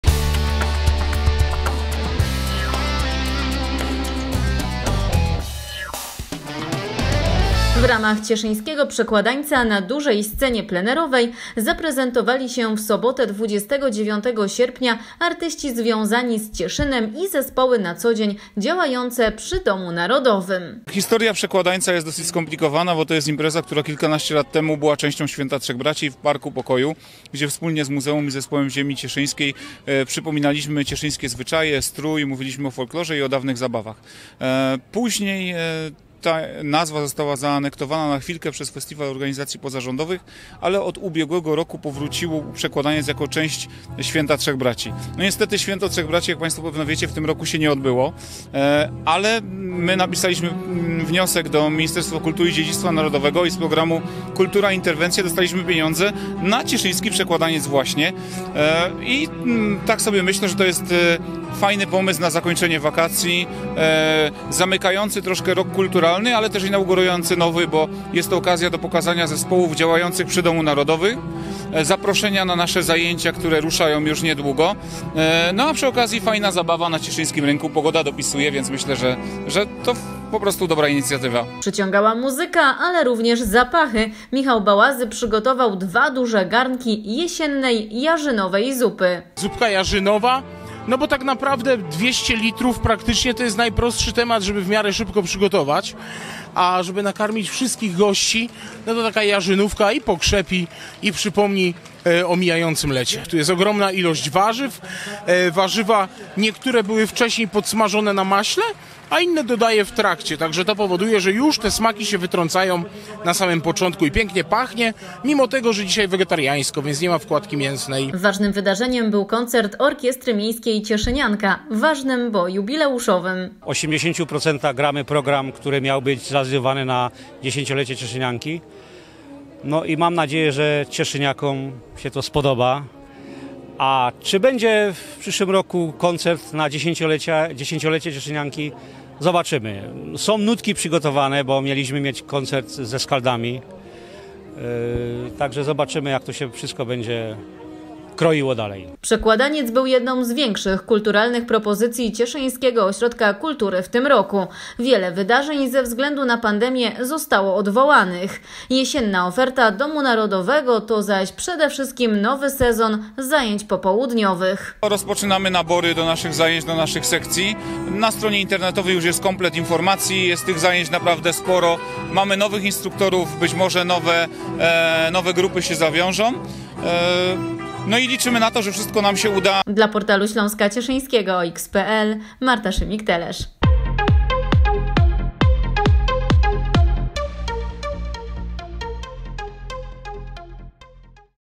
Cieszyński Przekładaniec za nami
Amatorska Grupa Gitarowa AGG, Stowarzyszenie Mażoretek "Cieszyńskie Gwiazdeczki", Zespół Pieśni i Tańca Ziemi Cieszyńskiej im. J. Marcinkowej , Zespół tańca żydowskiego "KLEZMER", Miejska Orkiestra Dęta "Cieszynianka", TEATR NA DŁONI, Michał Bałazy kulinarnie, a na zakończenie koncert "Cieszyn śpiewa Skaldów" - jednym słowem działo się! Miniony weekend (29.08) upłynął w Cieszynie pod znakiem Przekładańca. Zobacz relację z tego wydarzenia.